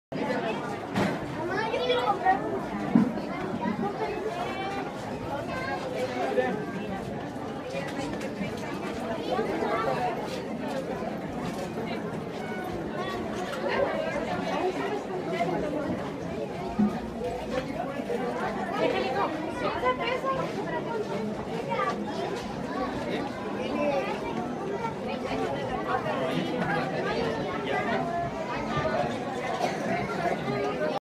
San Lucas Mich.
Visita al santuario de la virgen de guadalupe y aquí en una tienda de imágenes religiosas.